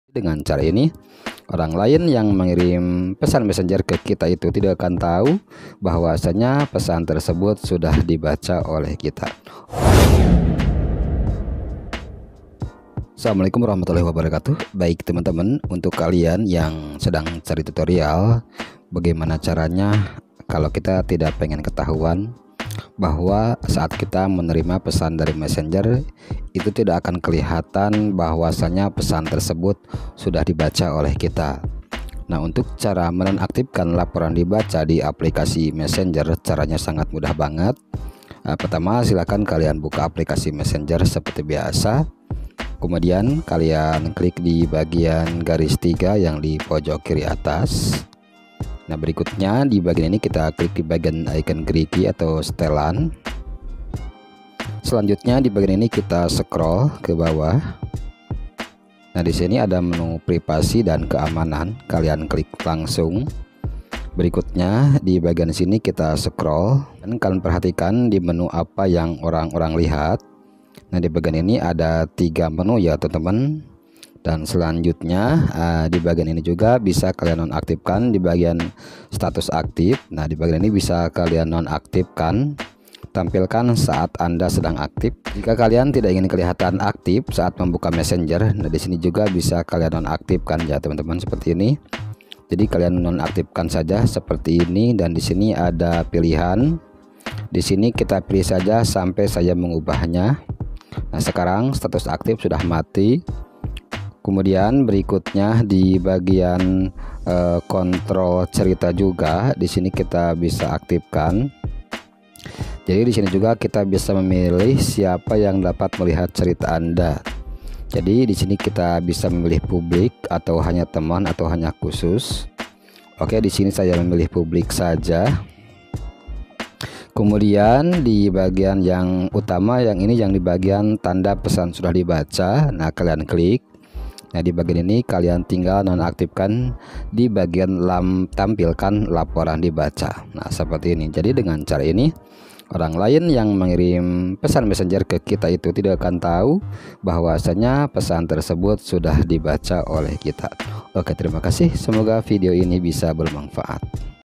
Cara menyembunyikan laporan sudah di baca di mesenger
Cara menyembunyikan laporan di baca di messenger.

kali ini utep channel bagikan tutorial cara agar tidak ketahuan oleh si pengirim kalau kita sudah baca pesan mesenger.atau Cara menyembunyikan laporan di baca di mesenger.

bagaimana cara menyembunyikan laporan di baca di messenger?
bagaimana cara sembunyikan laporan di baca di inbox?